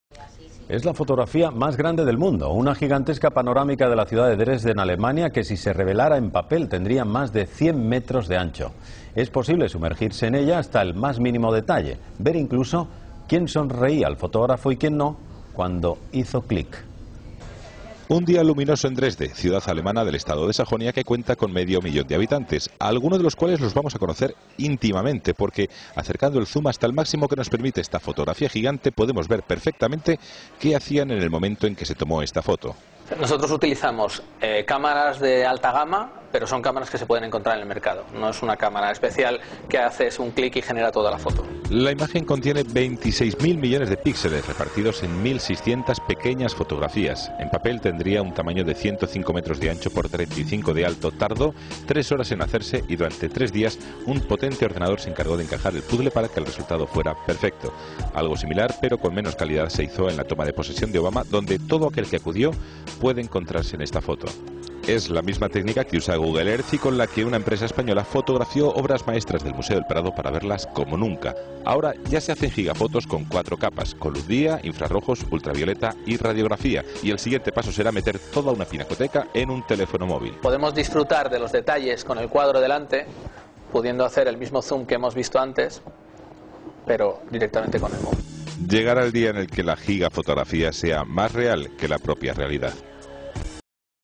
Television News - Antena 3 - Spanien
Antena 3 - Spanien: WORLD RECORD: The picture was made with the Canon 5D mark II and a 400mm-lens. It consists of 1.665 full format pictures with 21.4 megapixel, which was recorded by a photo-robot in 172 minutes. The converting of 102 GB raw data by a computer with a main memory cache of 48 GB took 94 hours. With a resolution of 297.500 x 87.500 pixel (26 gigapixel) the picture is the largest in the world. ( December 2009)